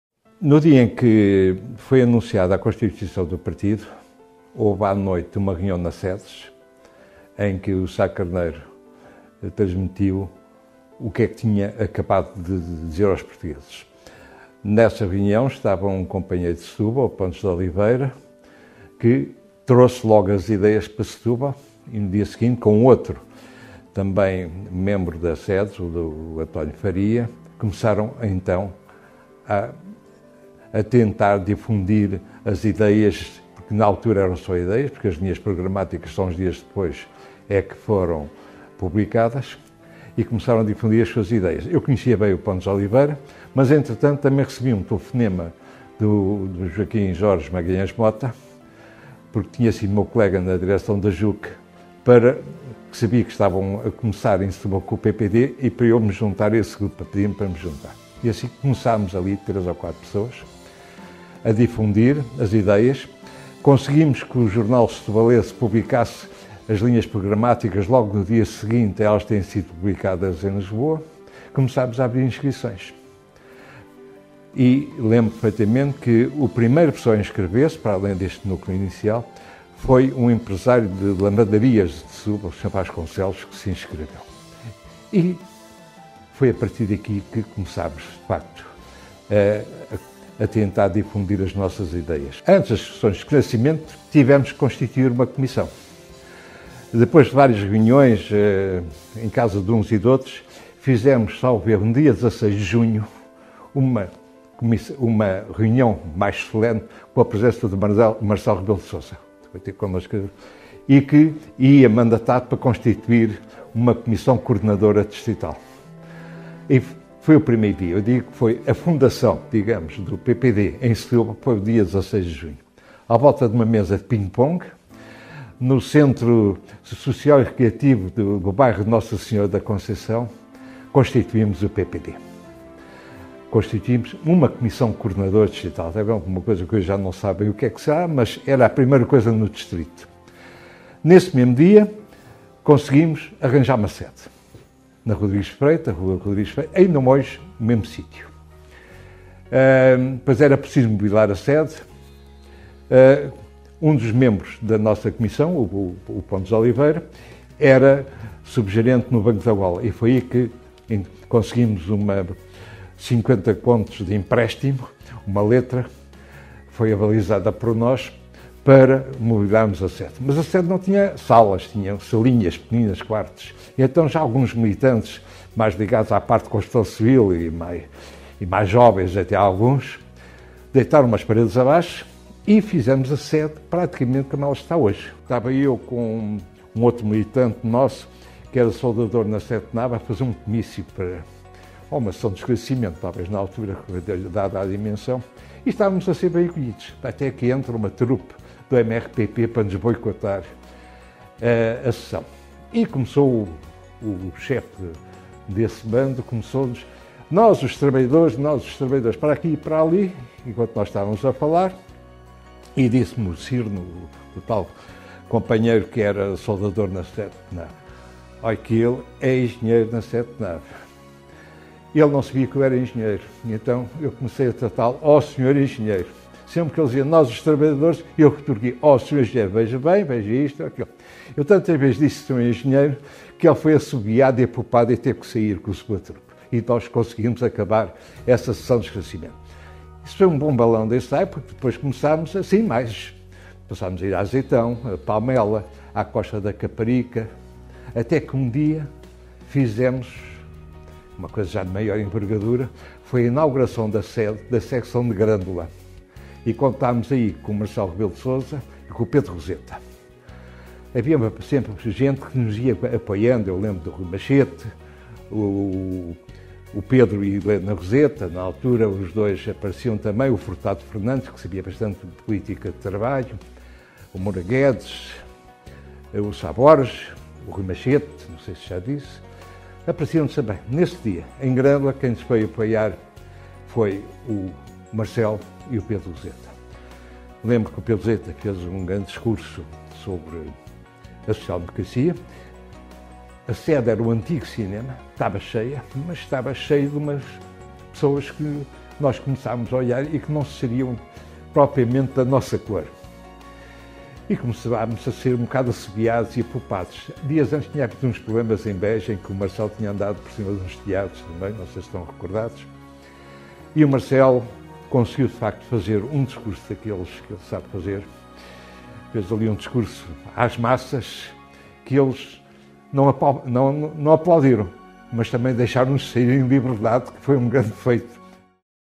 "À volta de uma mesa de ping pong, constituímos o PPD"
João Sá Nogueira, o militante mais antigo de Setúbal, conta os primeiros passos do PPD no distrito. Da divulgação das linhas programáticas à comissão coordenadora distrital.